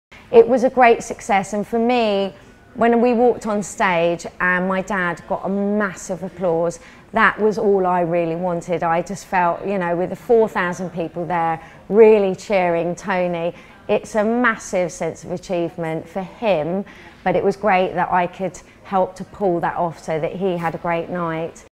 Inspiration: TONI&GUY Global Creative Director Sacha Mascolo Tarbuck on working with her father
Sacha Mascolo-Tarbuck discusses TONI&GUY's 50 years at the forefront of hairdressing and her father, Toni Mascolo's achievements.